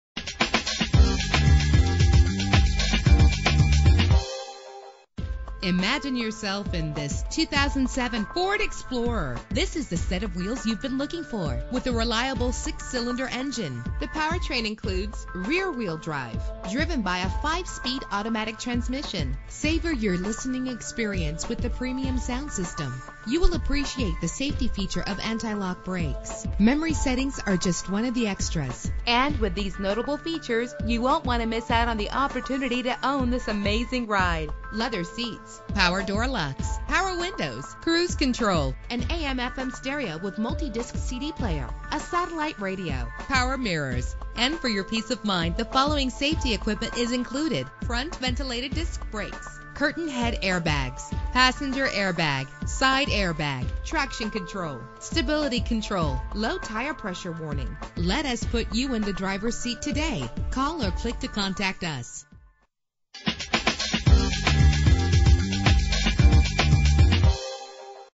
2007 Ford Explorer - Phoenix AZ
Year: 2007
Make: Ford
Model: Explorer
Trim: Limited
Engine: 4.0 liter 6 cylinder 12 valve
Transmission: 5-Speed Automatic
Color: White
Mileage: 67175
Address:
999 W Bell Rd